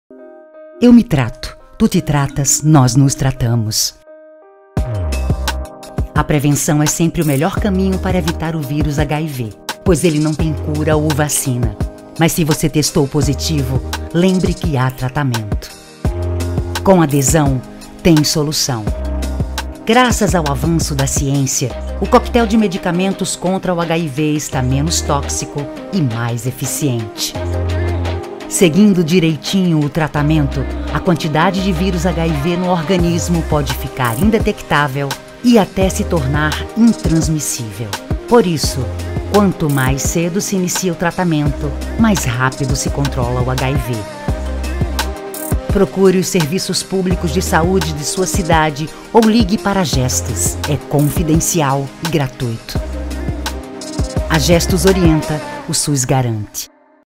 VT Campanha - Com adesão, tem solução
Eu me trato. Tu te tratas. Nós nos tratamos.

A prevenção é sempre o melhor caminho para evitar o vírus HIV, pois ele não tem cura ou vacina. Mas se você testou positivo, lembre que há tratamento. Com adesão, tem solução.

A Gestos orienta, o SUS garante.